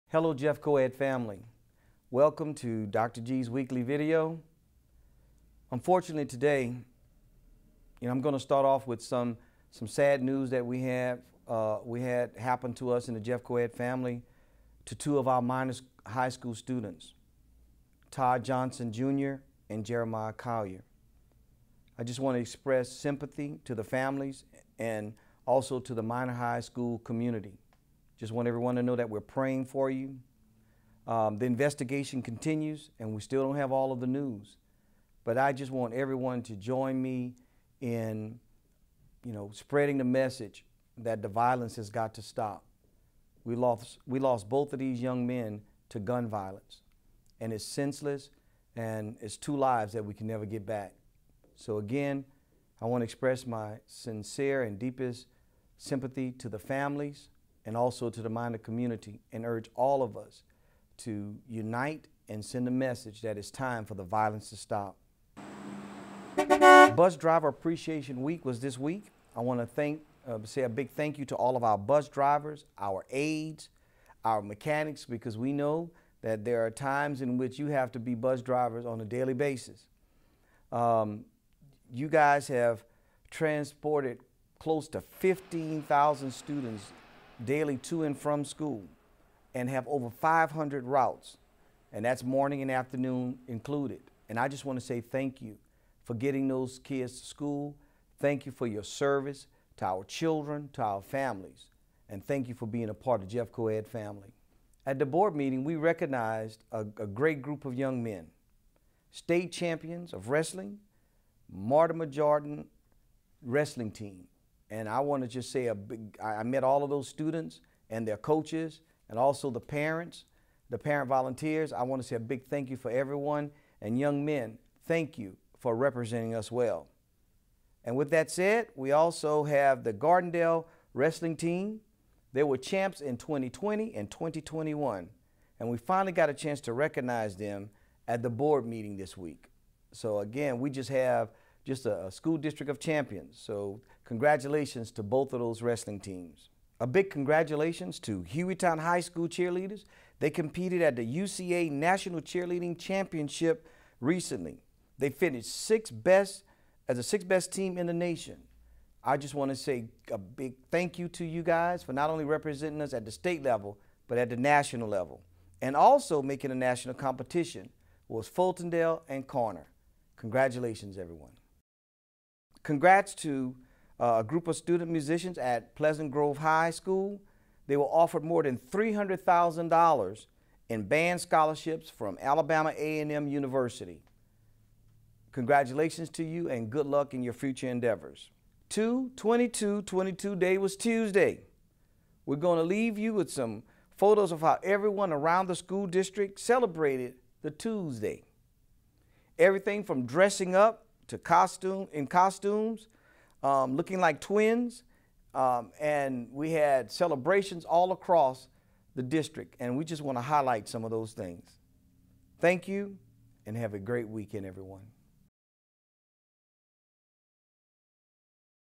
Weekly Update from Jefferson County Schools - 2/25/22
Weekly updates from the Jefferson County School District. This week we had TWOsday, Bus Driver Appreciation, recognition for our wrestling state champions, and more!